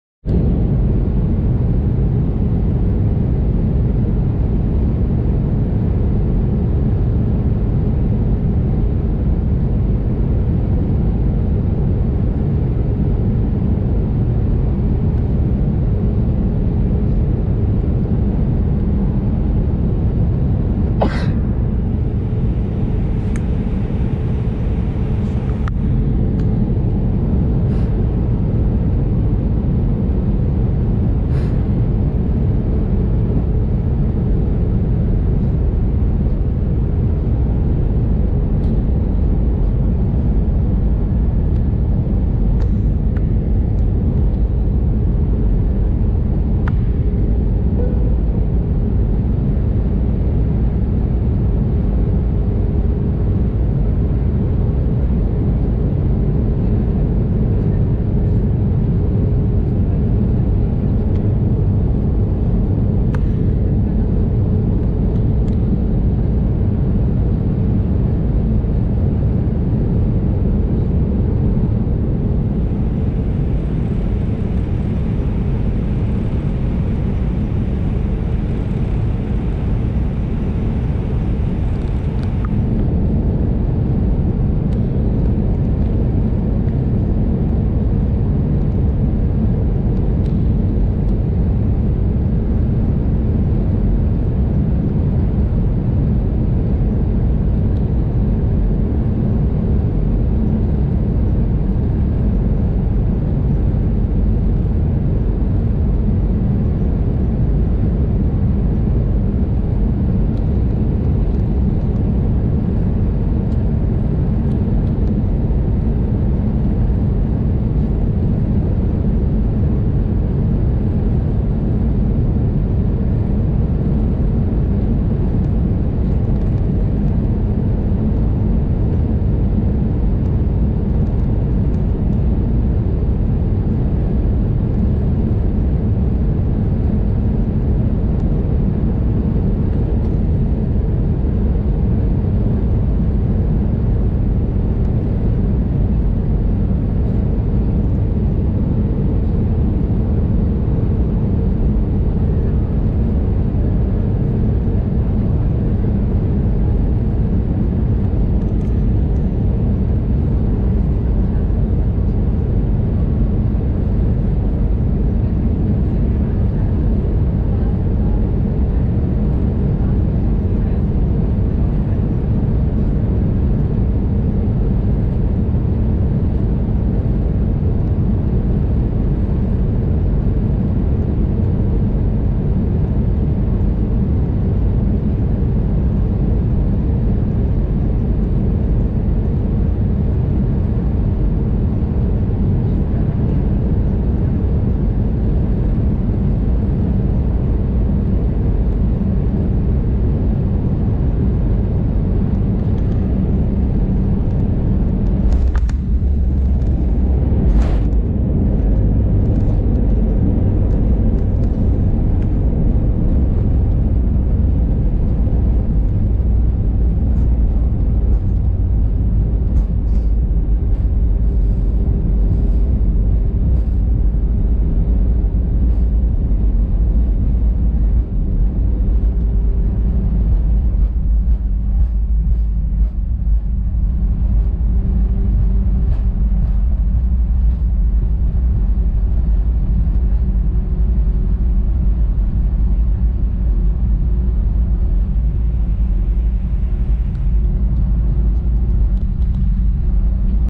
Porto Landing | Laudamotion Airbus A320
✈Route: VIE-OPO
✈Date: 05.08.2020
✈Airline: Laudamotion
✈Aircraft Type: Airbus A320-232
✈Duration: 2.55h
✈Seat: 6F
✈Registration: OE-LOY
✈Ticket Price: 21€